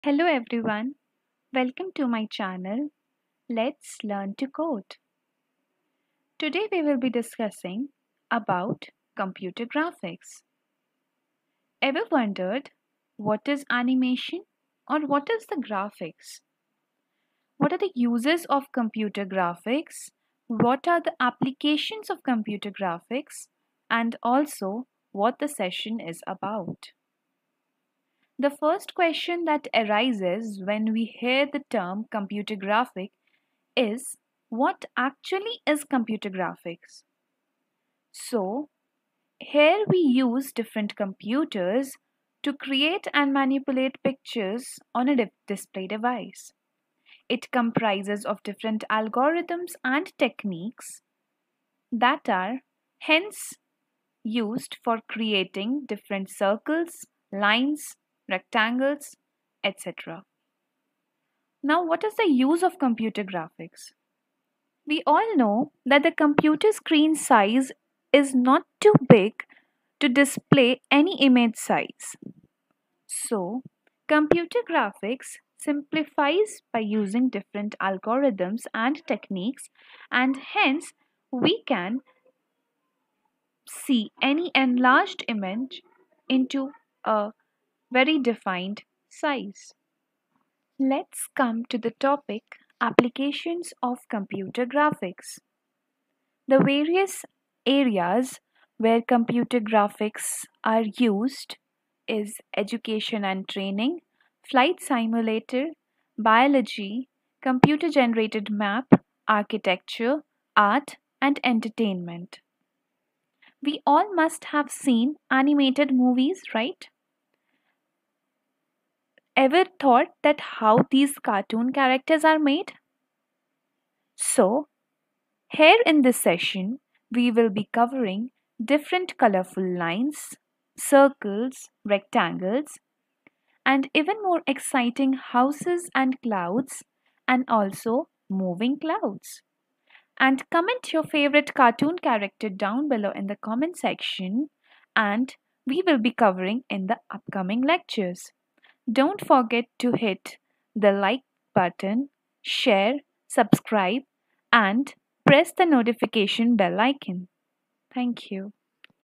Introduction to Computer Graphics | Let's Learn to Code
This is an attempt to impart my knowledge to the viewers who are excited about graphics, Animations in C language.
In the upcoming sessions I will be covering how to draw beautiful colored circles, lines, rectangle and it will be much more exciting when we will draw house and flying clouds.
Comment your favorite characters down below and draw the cartoon character along with me.